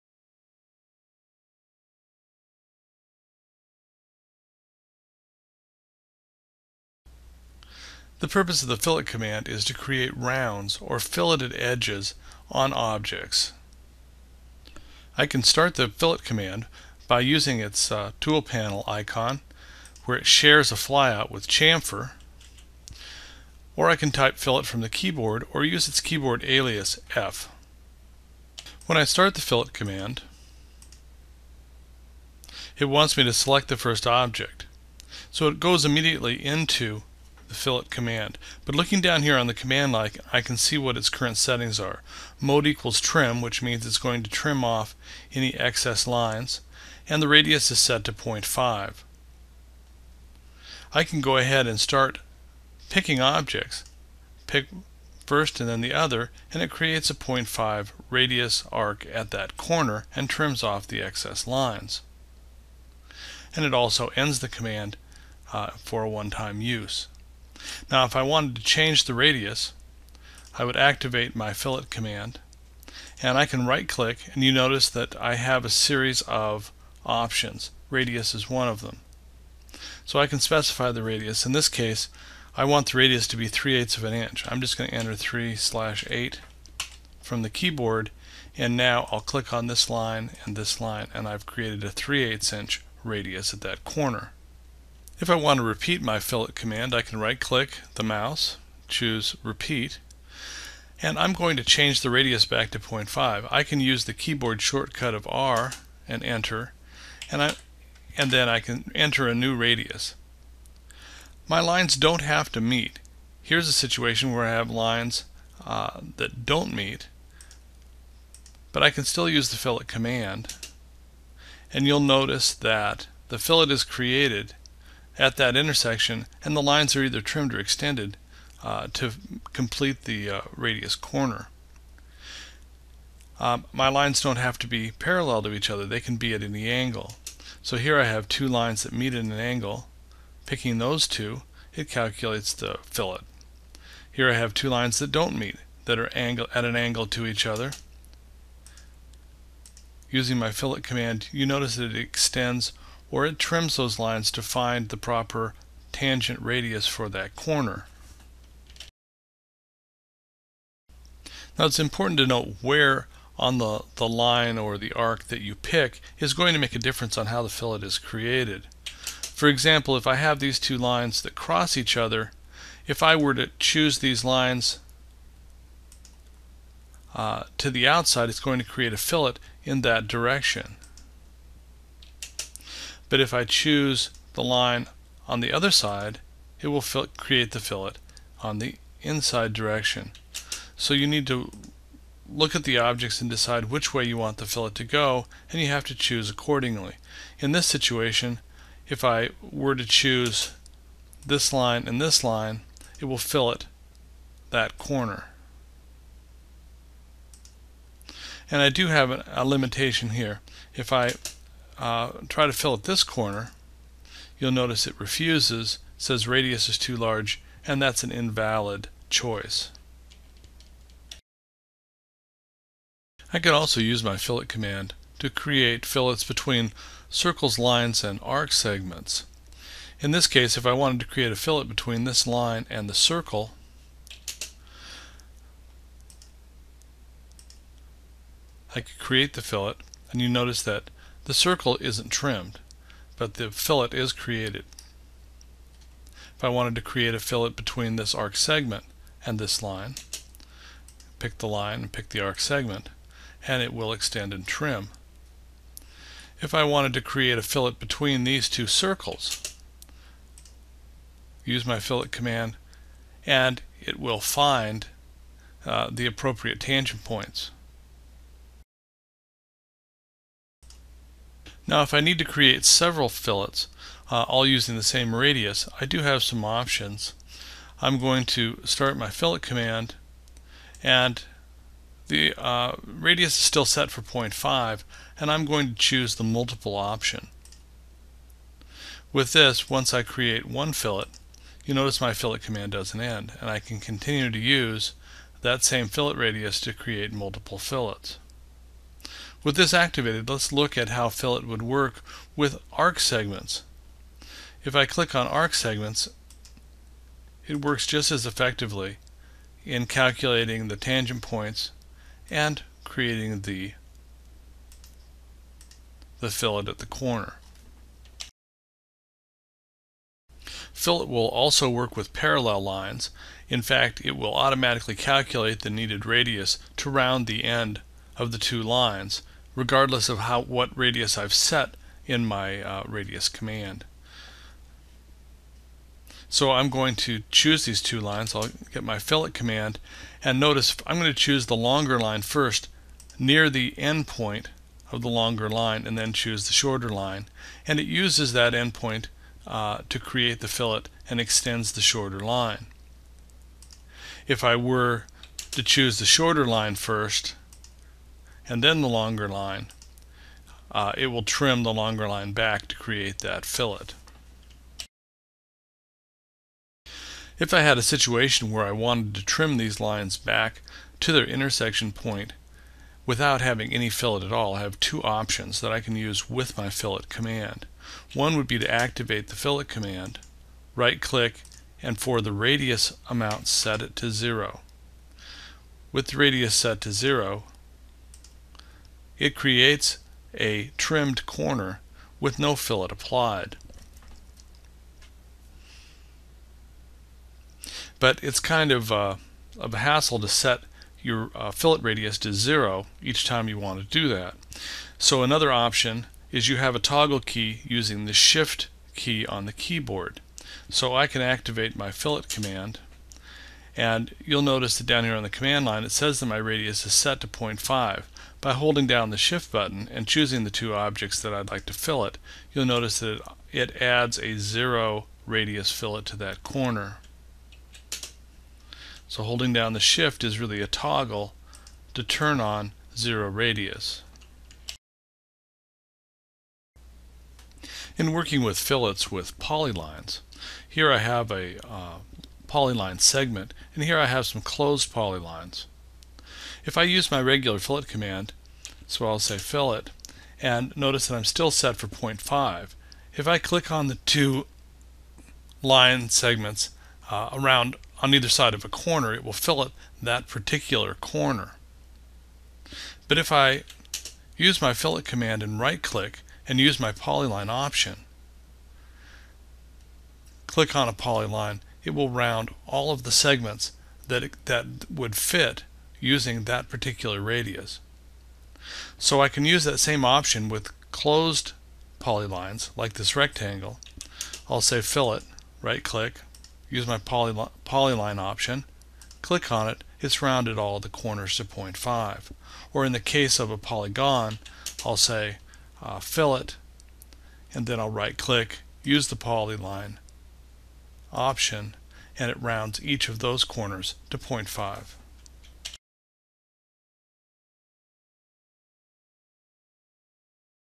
AutoCAD Tutorial for Beginners - Using the FILLET Command
The FILLET command will create rounded intersections of objects.  This allows you to round corners and create fillets to your chosen radius.  The FILLET command works with lines, arcs, circles and polylines.  With the radius set to 0, you can create or trim non filleted intersections.